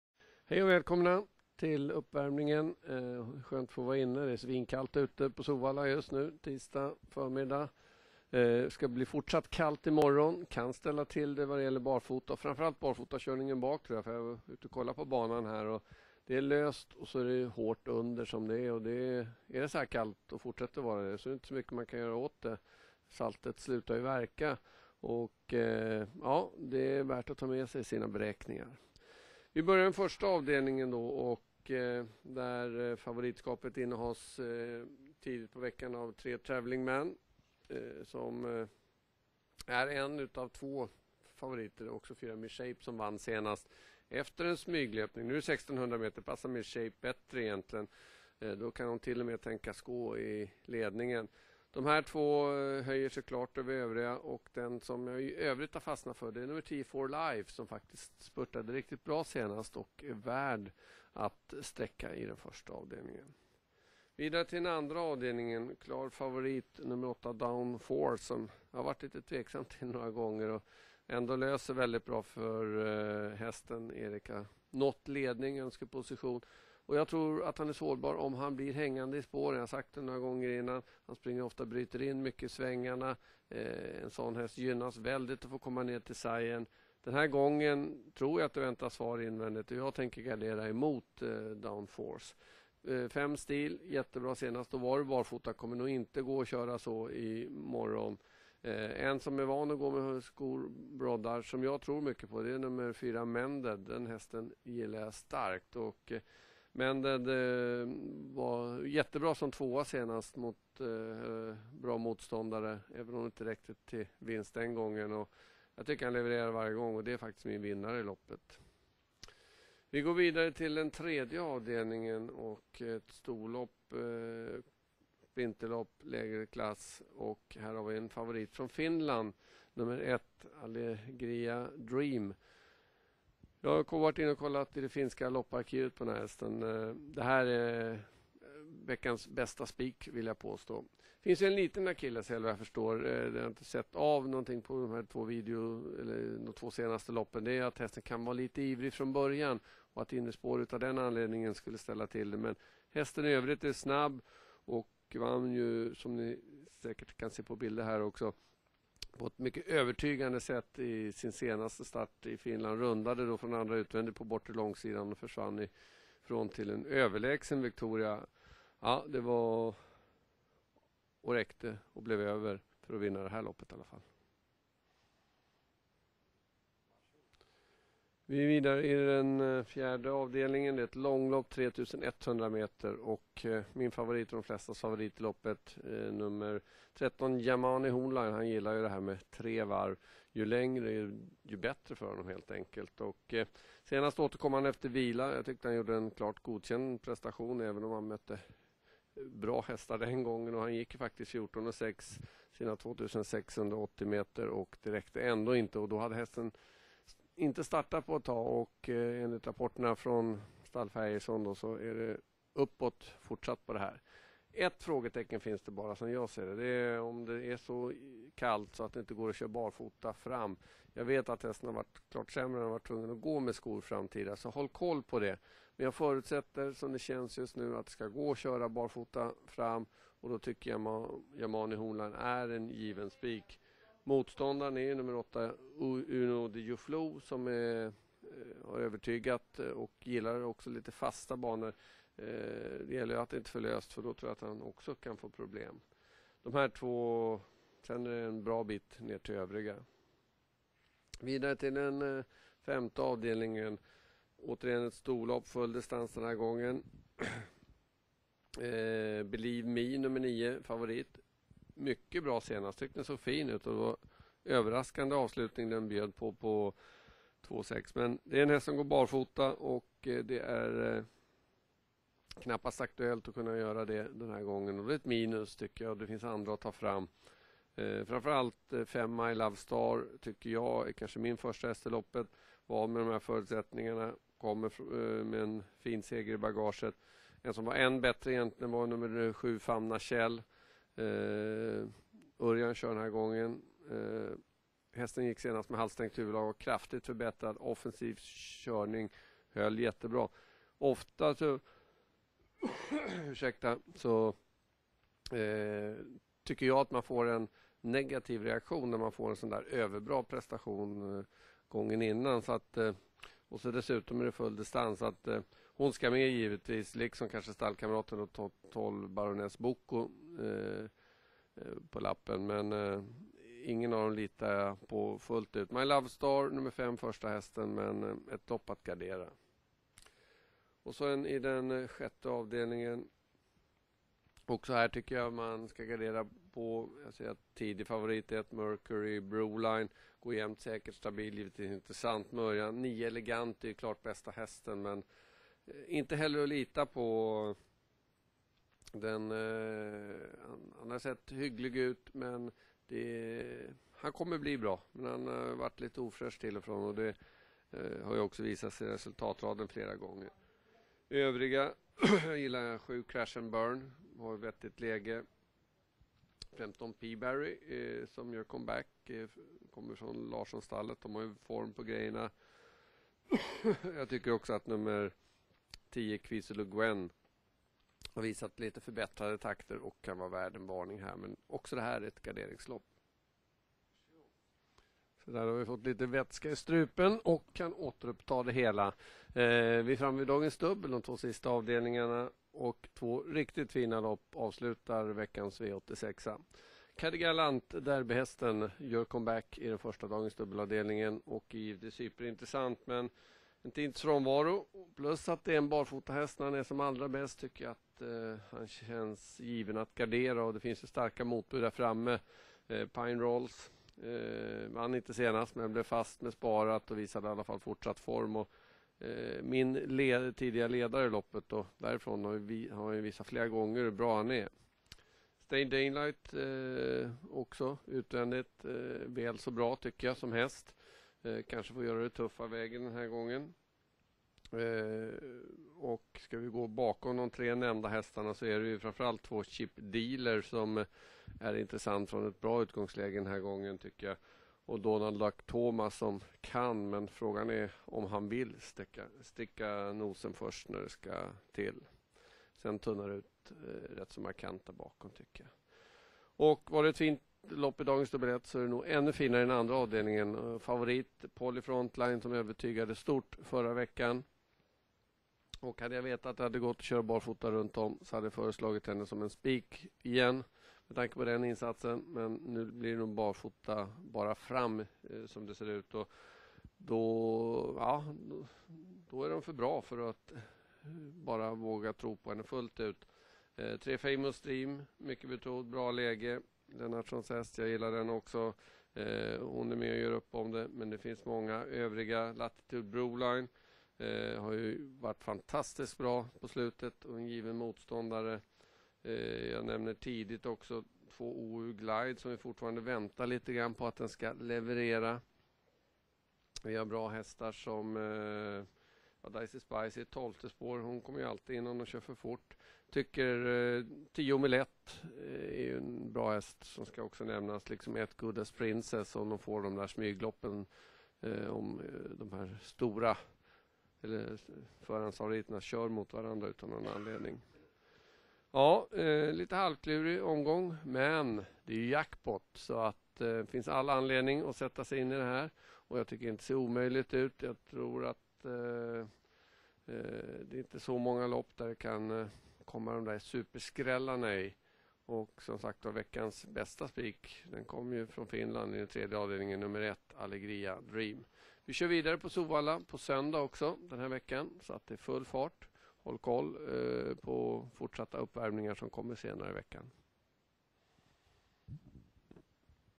UppWärmning med Widman130123
Förhandssnack med Fredrik Widman kring Solvallas V86, onsdag 23/1-2013